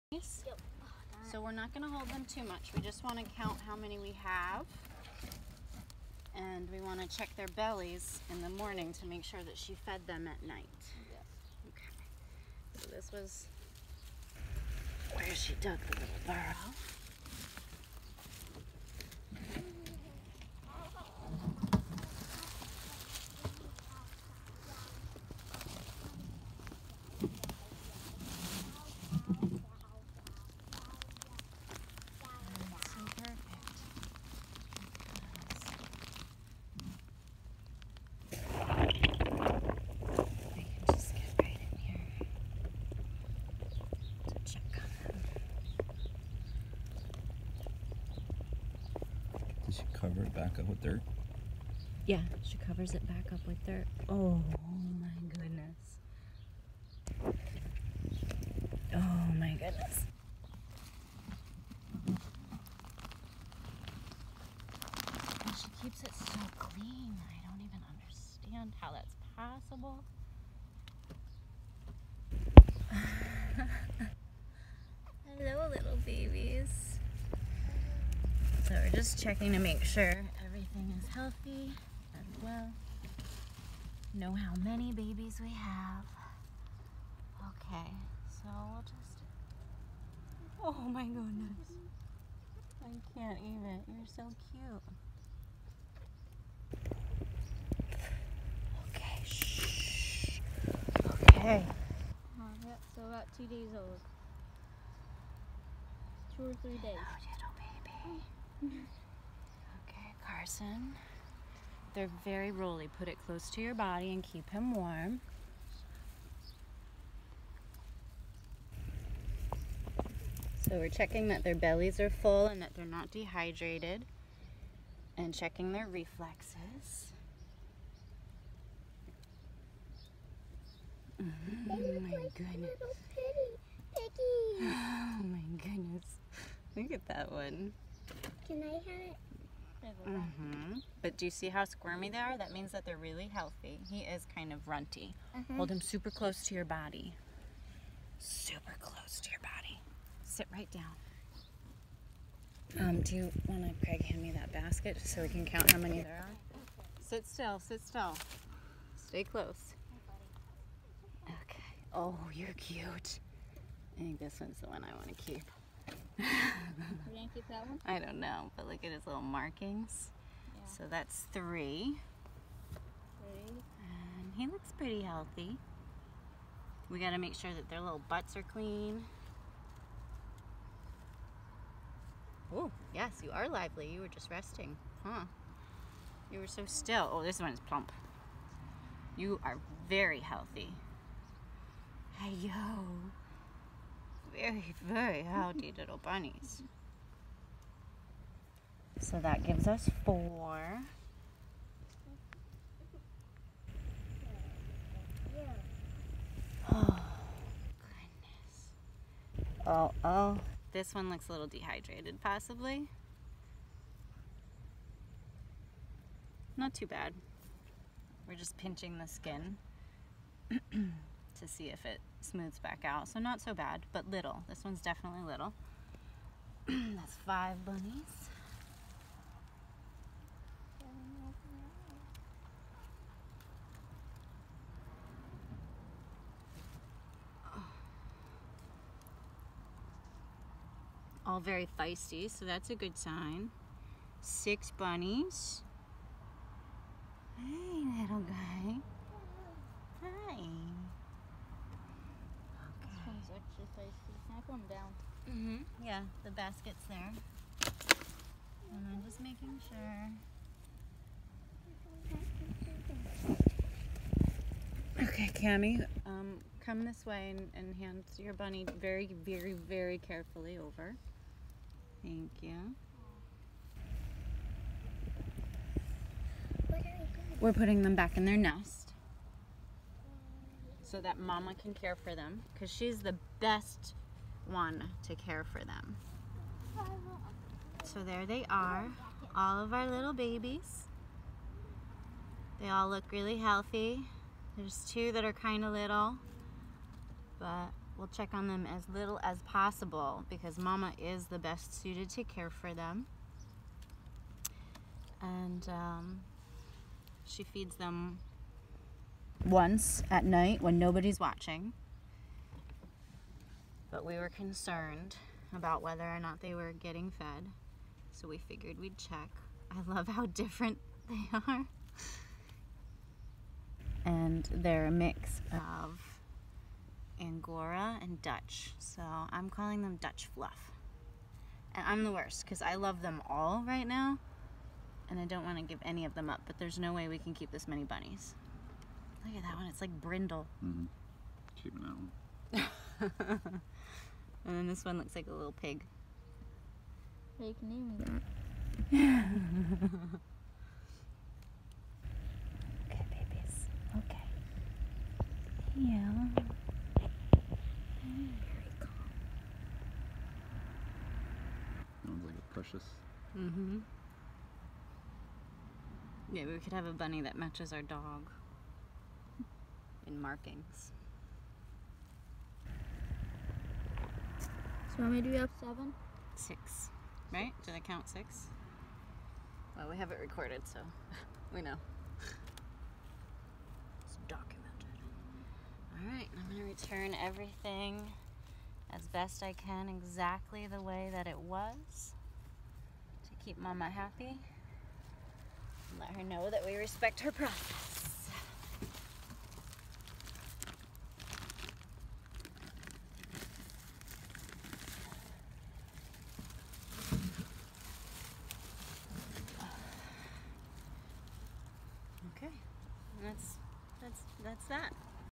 Day 2 bunny blog
I wasn’t originally excited that my daughter’s bunny may be pregnant. She didn’t get much bigger and it seemed as though she had covered up her little burrow so I figured we were in the clear. Then I began to notice patches of hair missing from her coat and the fur was no where to be found in her enclosure. We found the nest just on the other side of her enclosure and were surprised by the measures she takes to cover the borrow back up on her end. At first I was a little overwhelmed but I can see how much joy this experience is bringing to these kids right now and their joy can light up the world around them. Discovering gratitude in the process.